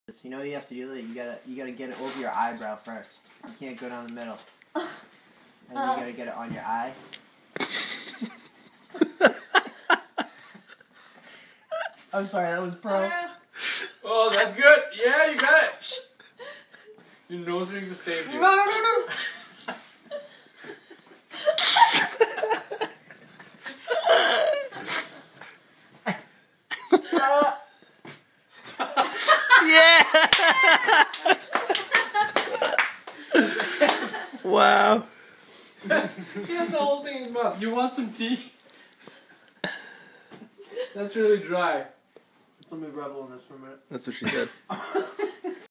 Cookie Face: Neil Edition
Practicing for the cookie face Olympics